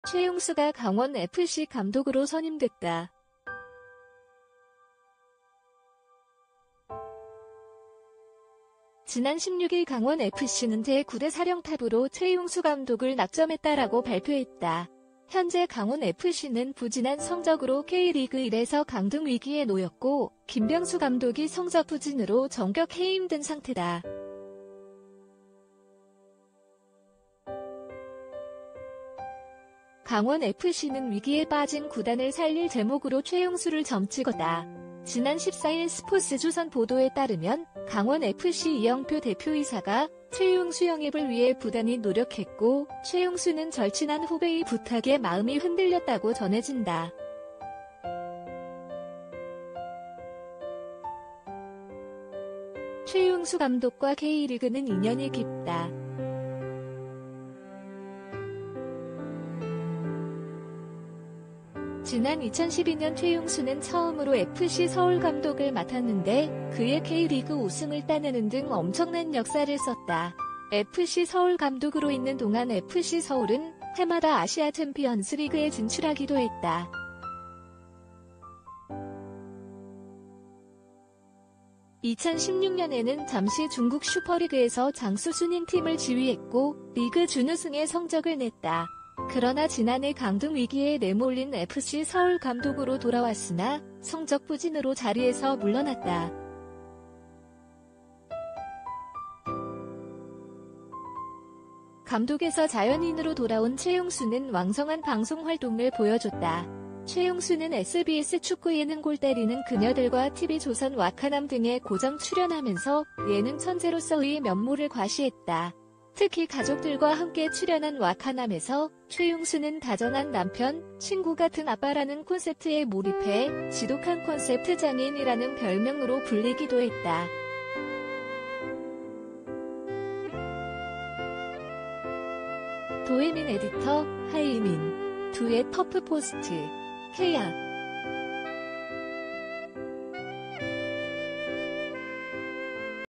이제는 '예능 천재' 된 최용수가 전한 뜻밖의 근황은 사실 당연한 결말이다오피셜이제는 '예능 천재' 된 최용수가 전한 뜻밖의 근황은 사실 당연한 결말이다(오피셜)
최용수가 강원FC 감독으로 선임됐다.
지난 16일 강원FC는 ”제9대 사령탑으로 최용수 감독을 낙점했다”라고 발표했다. 현재 강원FC는 부진한 성적으로 K리그1에서 강등 위기에 놓였고, 김병수 감독이 성적 부진으로 전격 해임된 상태다.
강원FC는 위기에 빠진 구단을 살릴 재목으로 최용수를 점찍었다. 지난 14일 스포스조선 보도에 따르면 강원FC 이영표 대표이사가 최용수 영입을 위해 부단히 노력했고, 최용수는 절친한 후배의 부탁에 마음이 흔들렸다고 전해진다.
최용수 감독과 K리그는 인연이 깊다.
지난 2012년 최용수는